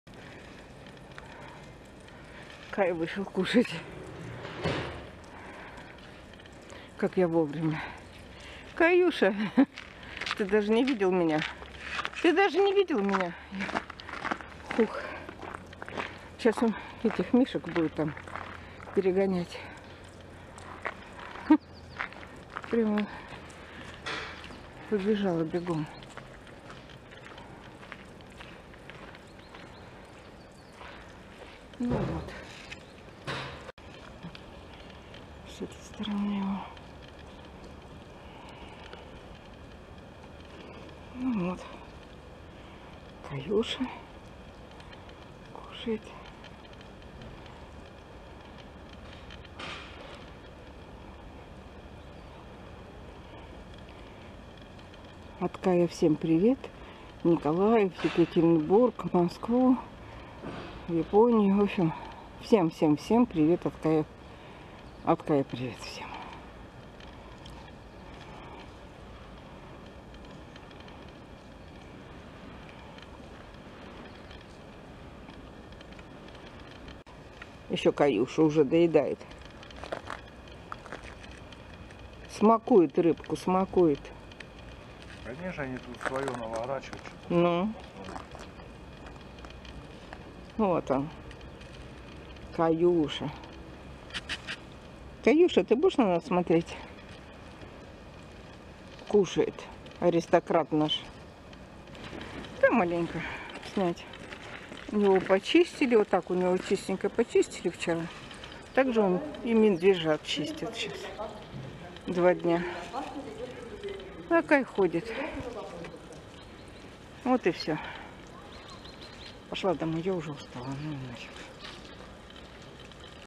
Завтрак у Кая на улице 09.11.2023
Кай (Красин) родился в Санкт-Петербурге 27 ноября 2007 года. В Новосибирск переехал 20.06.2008. Мама Услада, папа Меньщиков.
Второй по величине сухопутный хищник планеты после гребнистого крокодила. Масса животного может достигать 800 кг. Средний вес самца 400—450 кг, длина тела 200—250 см. Самки заметно мельче: средний вес 350—380 кг, длина тела 160—250 см. Высота в холке 130—150 см. Подошвы ног покрыты шерстью, что позволяет им не скользить по льду и не мёрзнуть. Между пальцами есть плавательная перепонка, а передняя часть лап оторочена жёсткими щетинками. Крупные когти могут удержать даже сильную добычу.
Одиночные животные. Как правило, миролюбивы по отношению друг к другу, но между самцами в брачный период случаются стычки. Взрослые самцы могут нападать на медвежат.
Гон с марта по июнь. Продолжительность жизни — максимум 25—30 лет; в неволе рекорд долгожительства — 42 года.
В зоопарке медведей кормят утром морской рыбой, хлеб с рыбьим жиром внутри, морковь, яблоки, морская капуста. Медвежат подкармливают молоком из соски. На ужин мясо (говядина, конина, куры или индейка). Летом и осенью арбузы, дыни, ранетки, кабачки, тыквы. Когда показательное кормление летом бросают живую рыбу.